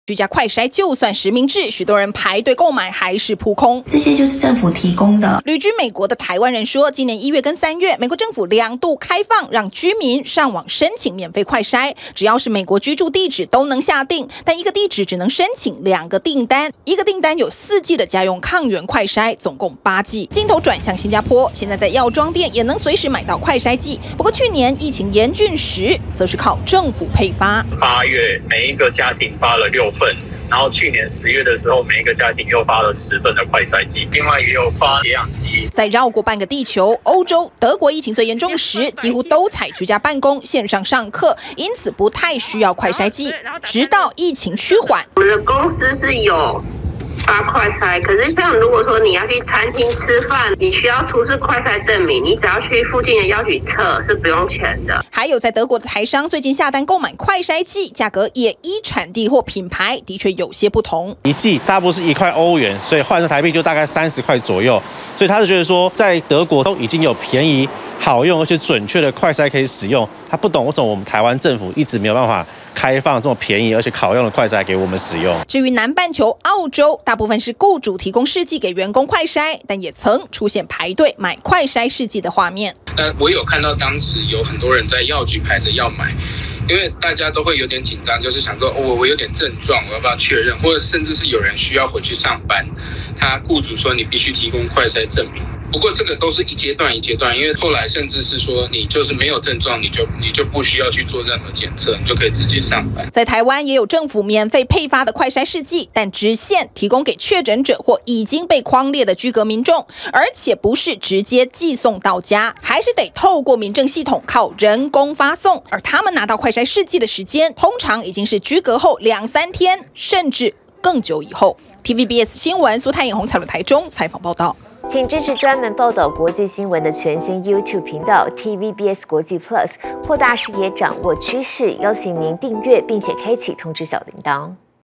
台灣難買! 美國.新加坡「免費寄快篩劑到府」｜TVBS新聞
新增本土確診個案連日破萬人，疫情大爆發，卻有很多需要居家快篩的民眾買不到快篩劑，來看看其他國家狀況，美國在今年初只要是居民，就能依照地址申請免費快篩劑，新加坡政府在去年也兩次寄發免費快篩劑給民眾，德國則有不同價格與品牌的快篩劑可以選擇，有人買到一劑一歐元的試劑，但目前台灣實名制的快篩試劑，一劑一百元，而且很難買到!